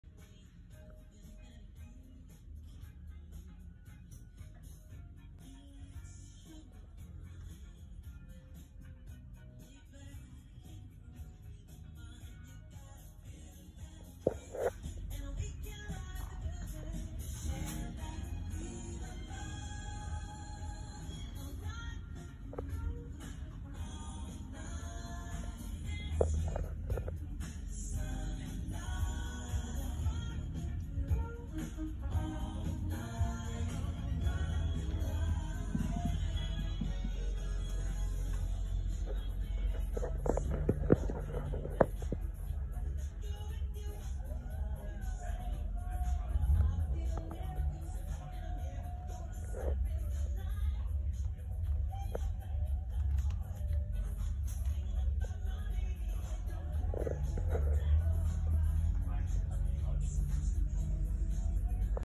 This was at the ozrc hobby shop
At the ozrc hobby shop and they had lots of RC upstairs at the ozrc hobby